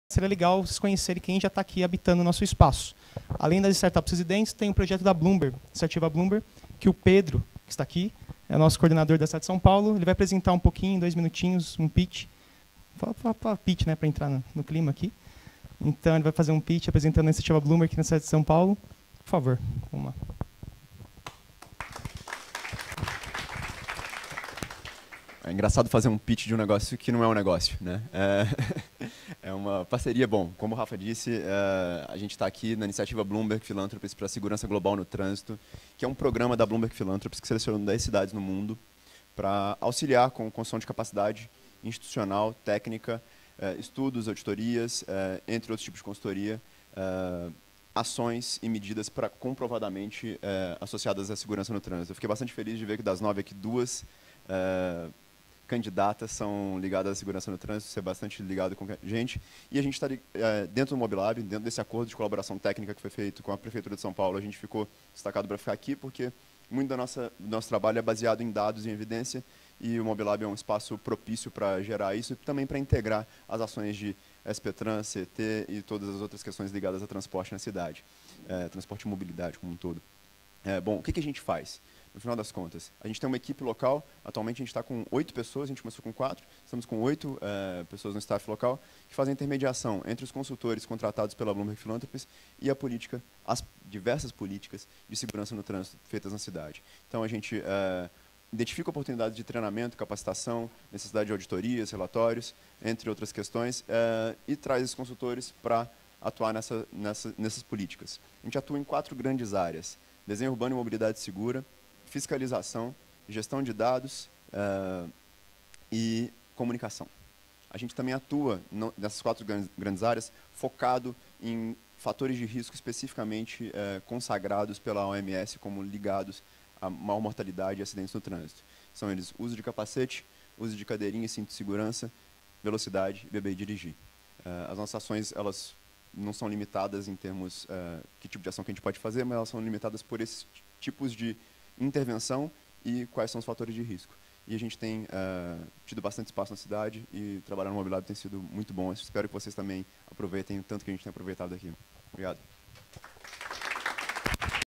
Iniciativa Bloomberg para a Segurança Global no Trânsito
A Iniciativa Bloomberg para a Segurança Global no Trânsito apoia a Prefeitura de São Paulo na implementação de intervenções baseadas em boas práticas de segurança viária e visando a redução de lesões e mortes no trânsito. Além da capital paulista, o projeto atua em outras 10 cidades ao redor do mundo: Fortaleza (CE), Bogotá (Colômbia), Acra (Gana); Adis Abeba (Etiópia), Bandung (Indonésia), Bangkok (Tailândia), Ho Chi Minh (Vietnã), Mumbai (Índia) e Xangai (China).